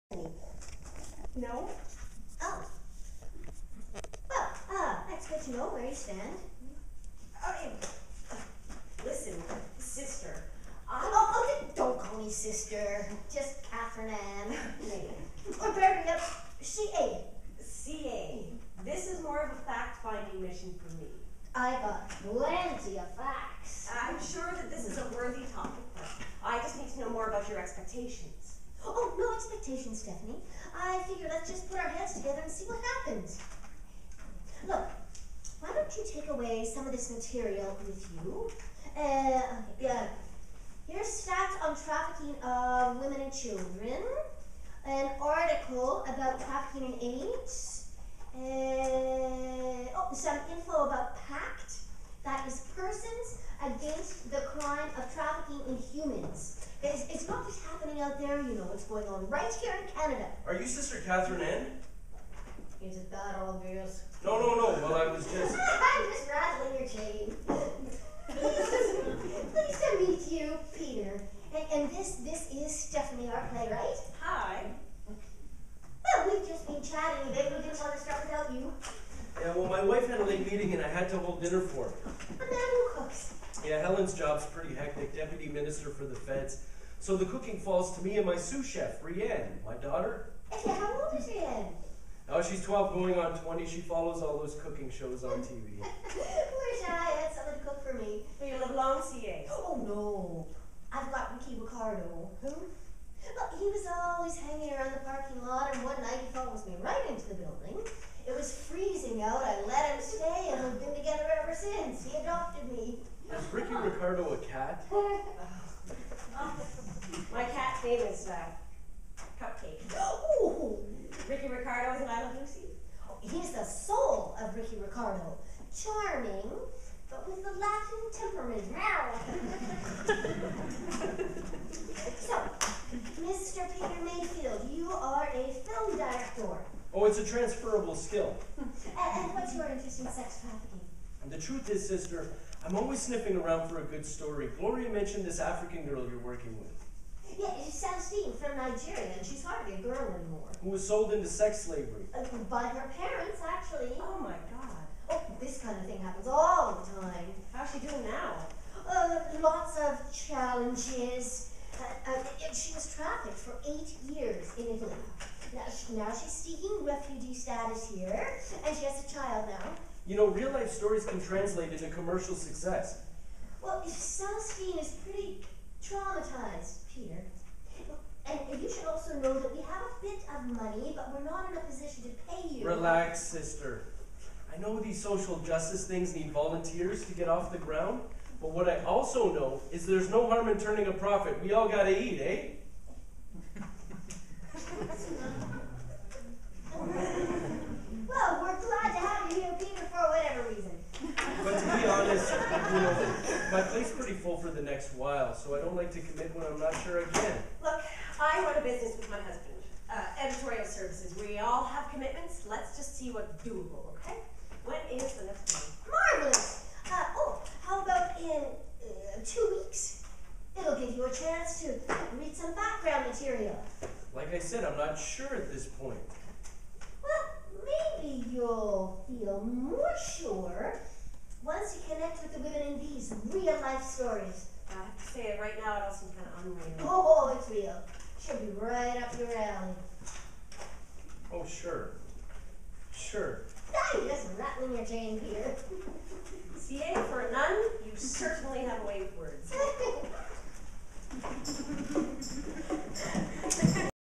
"The Walk" (pt 2) -- Women's Worlds 2011
"The Walk" - a play about sex trafficking written by Catherine Cunningham-Huston and produced by Moon dog Theatre in association with PACT-Ottawa (Persons Against the Crime of Trafficking in Humans) - features an unlikely trio trying to write a play about sex trafficking, but haunted by the stories surrounding it.

8pm, 4 July, Freeman Hall, Perez Building, University of Ottawa

Video shot by our fantastic volunteer Cheryl Gain.